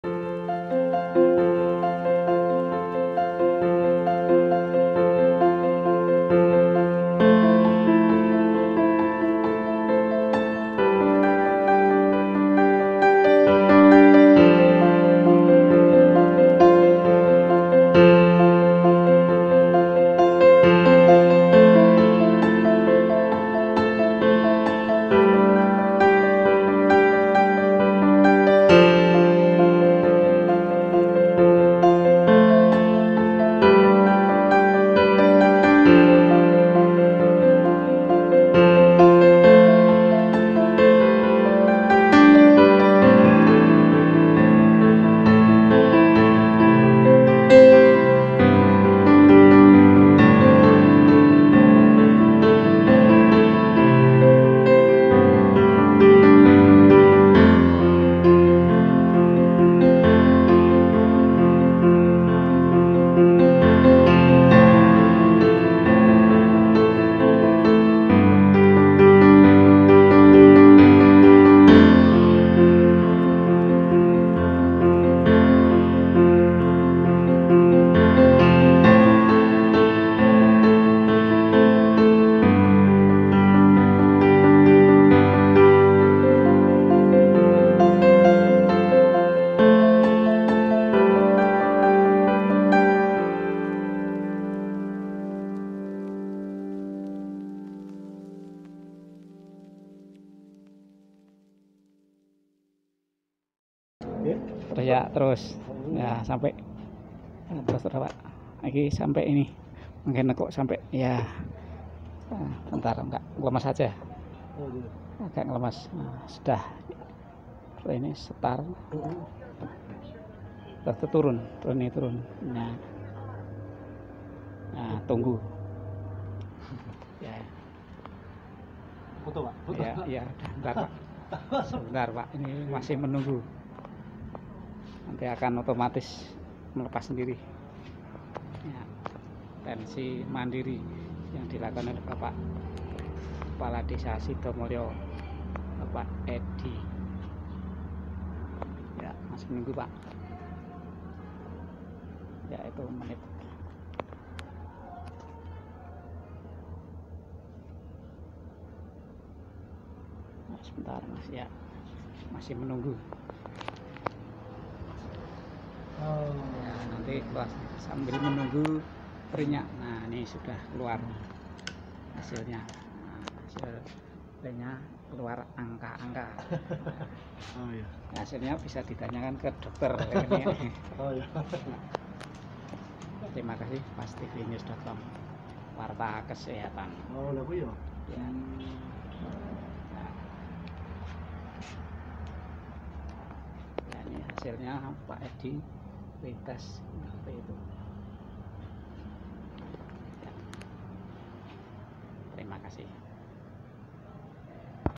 Tester Tekanan Darah Otomat di Puskesmas Bambanglipuro Bantul
Redaksi media pastvnews.com, salah ssatu alat kesehatan yang satu ini adalah alat untuk tes teknan darah yang hampir  ada di selurih puskemas sehingga warga bisa memanfaatkan alat secara mandiri.

Nah jika anda mau tes mandiri maka bisa melakukannya,
Caranya masukan tangan di lubang alat tersebut hingga siku tangan bisa di tekuk melebih sekat masuk.  selanjutnya tangan di lepas pelan dengan cara tangan terbuka .

 setelah ini maka cek lampu jika letak tangan sudah pas maka pantau lampu let warna hijau kemudina tekan tombol star.

Langkah se;lanjutnya tunggu hingga beberapa menit atau detik karena alat akan mendeteksi tangan kanan anda .
proses menunggu ini memerlukan kesabaran setelah itu hasil bisaa di lihat dan akan keluar kertas yang akan menunjukan hasilnya

jika kertas keluaran maka cek hasilnya jika SYS KISARAN 130 S/D 140 maka hasil tes normal, namun jika lebih dari 150 atau lebih maka bisa di bilang memiliki potensi tinggi. untuk lebih jelasnya hasil tes bisa anda tanyakan ke putugas Puskemas.

Kebetulan pada hari sabtu 21 maret  2020 di ldepan pintu masuk puskesmas Bambanglipuro Bantaul awak media bersama lurah desa Sidomuluo dan melakukan tes mandiri lewat alat tersebut. 'tim red